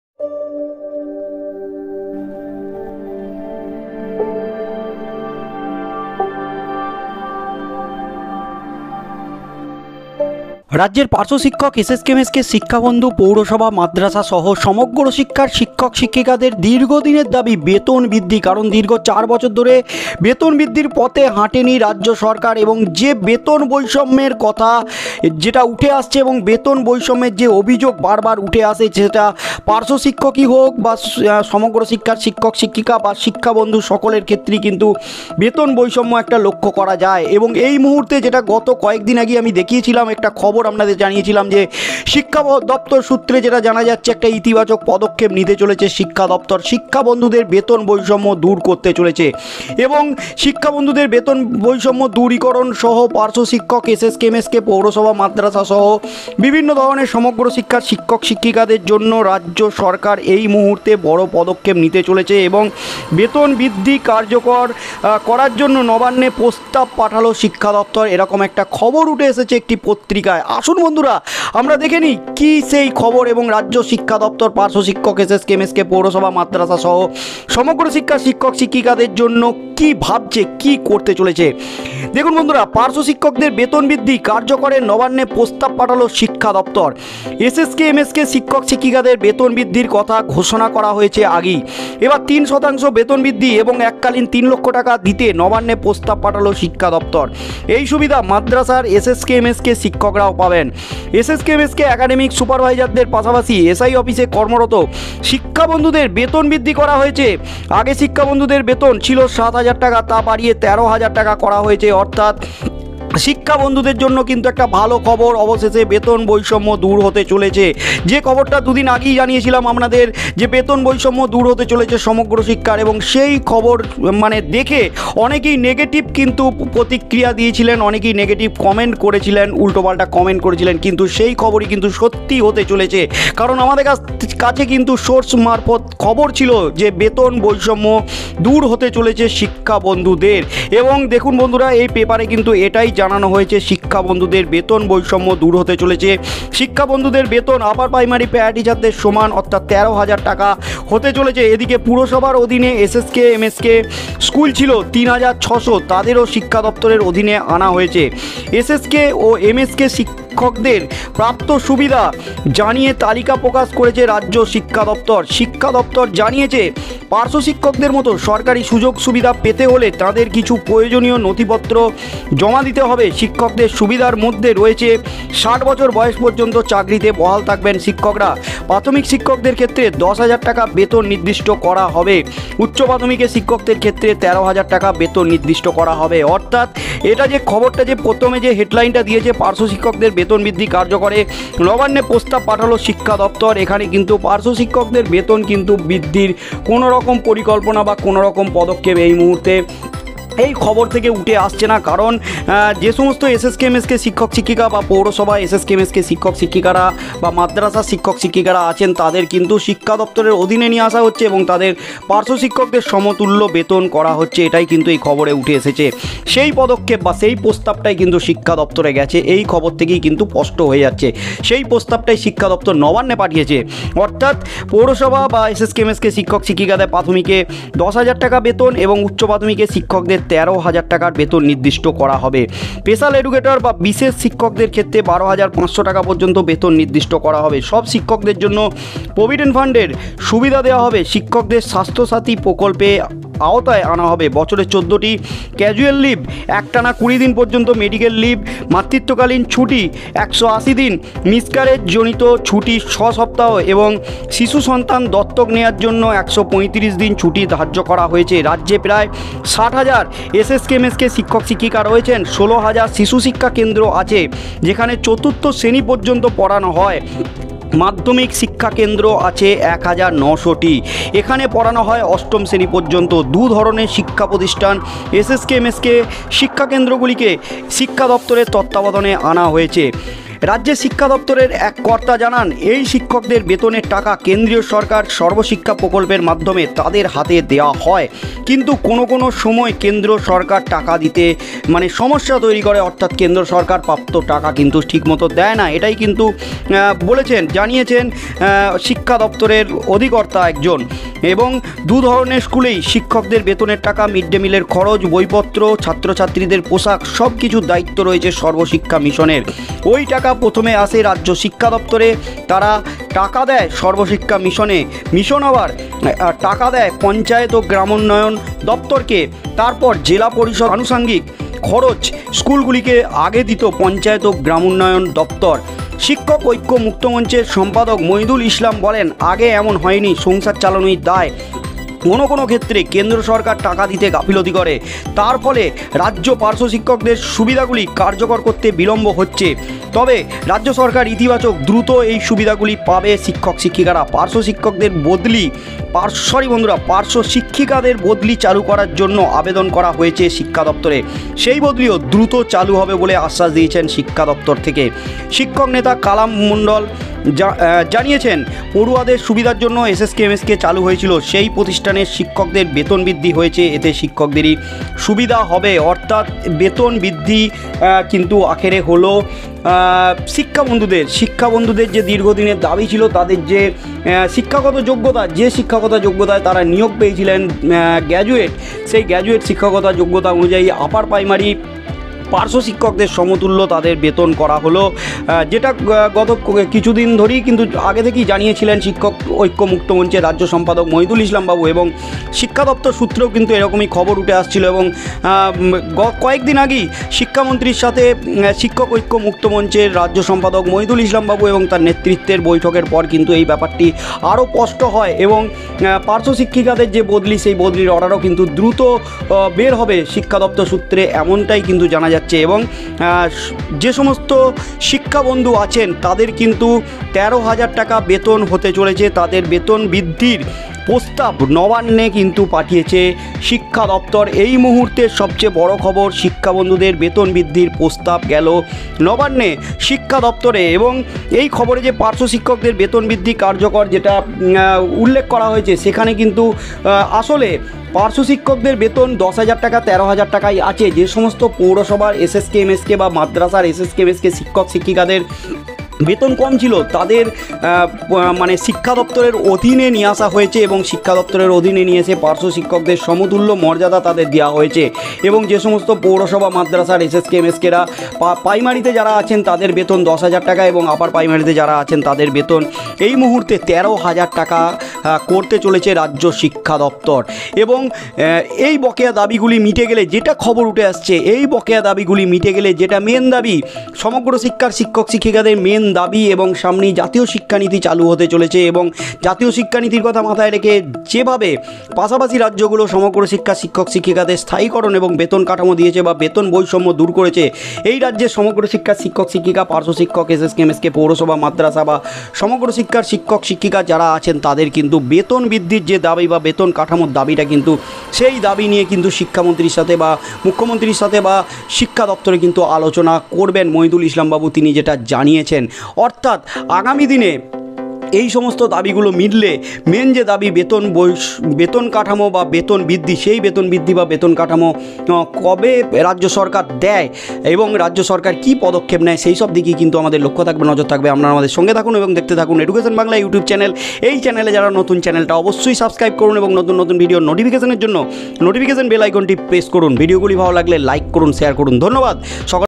বড় খবর🔴 সমগ্রশিক্ষায় বেতনবৃদ্ধি কার্য্যকর সহ একগুচ্ছ প্রস্তাব নবান্নে পাঠালো শিক্ষাদপ্তর/বড় সুখবর
ধন্যবাদ

Keyword⏩